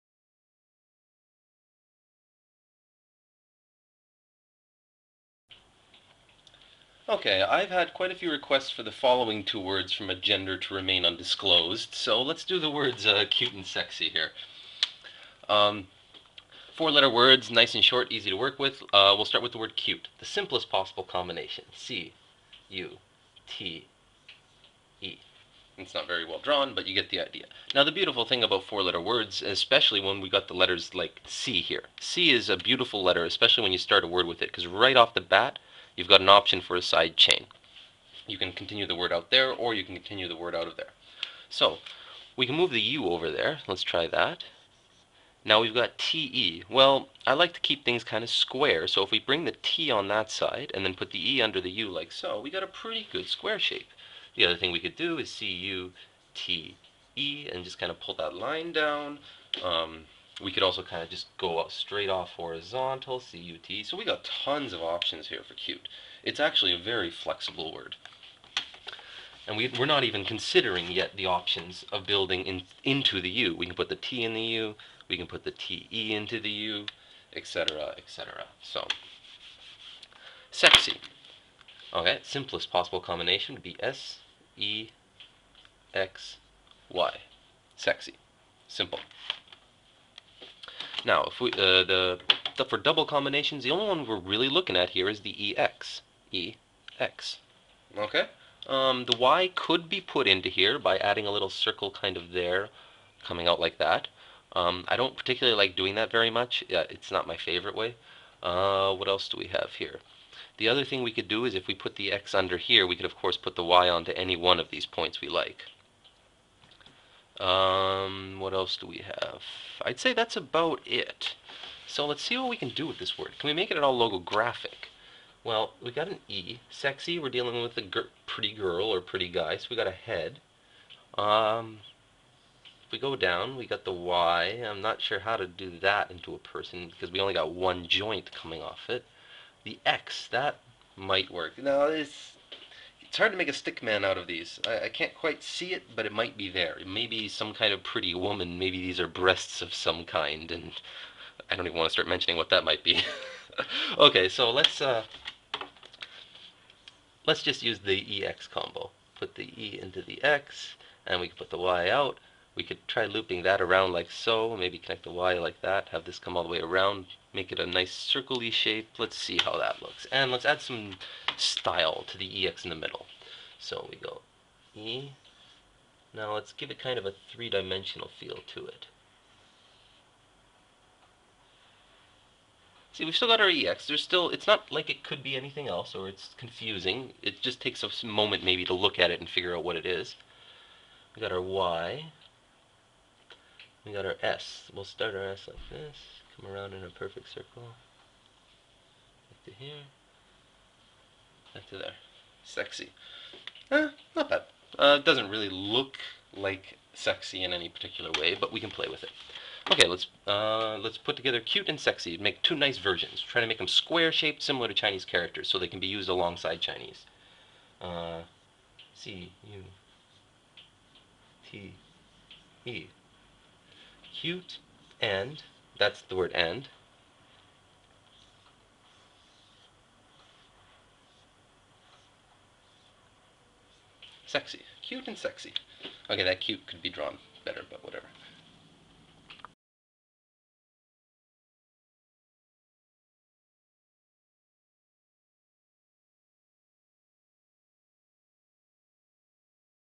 Cute n' Sexy Design Video - Dscript Text Art Lessons
For the girls, these are the 2 words you asked for most. Here is how to draw some of versions of the words cute and sexy. there amny other possible combinations other than the ones shown, to learn avbout dscript, visit dscript.ca